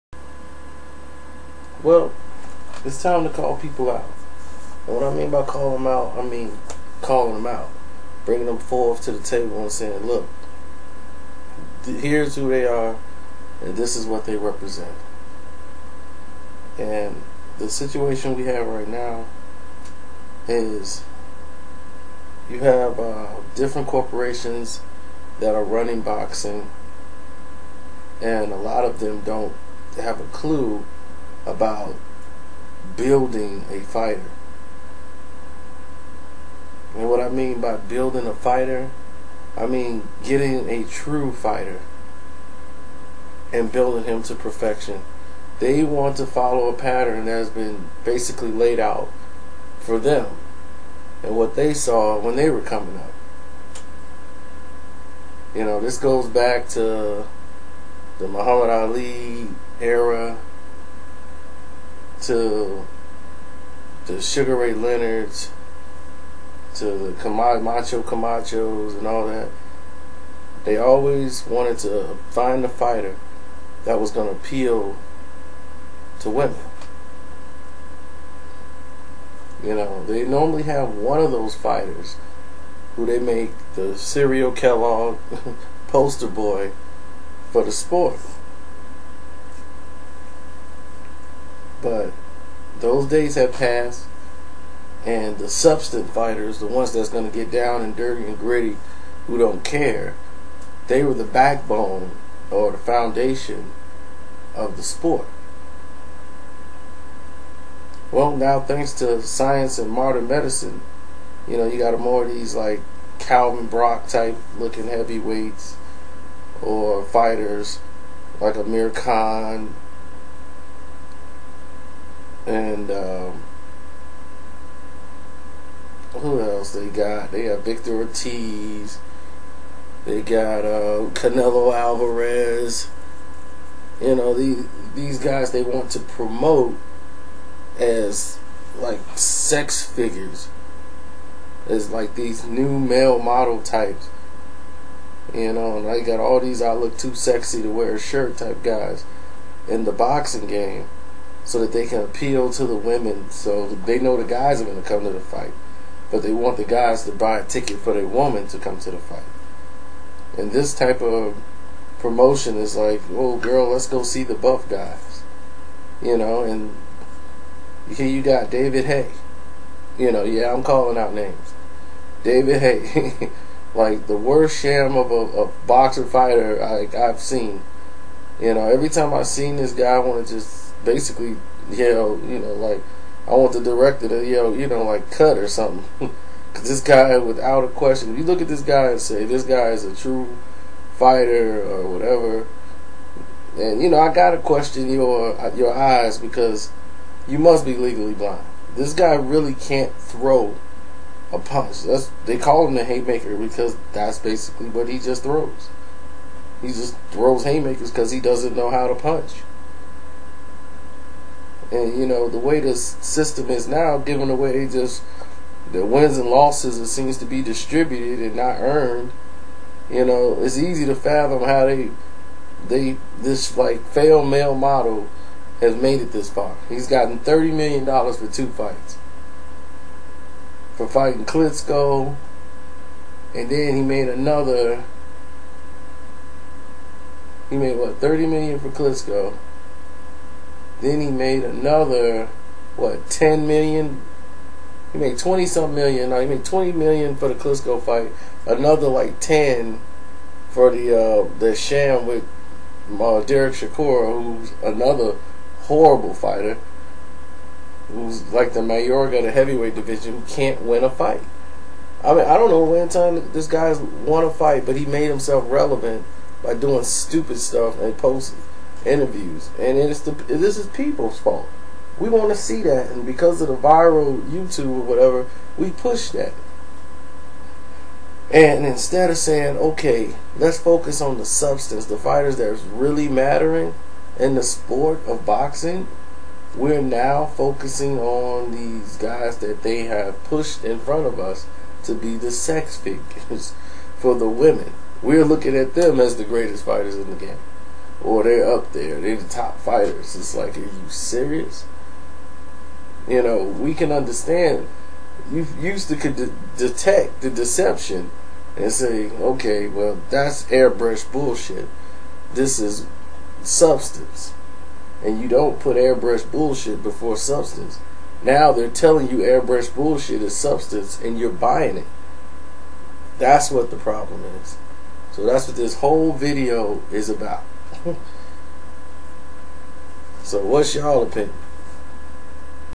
ARE BOXING PROMOTERS SELLING YOU SEX SYMBOLS OR FIGHTERS?
Fighters nowadays are looking like failed runway models.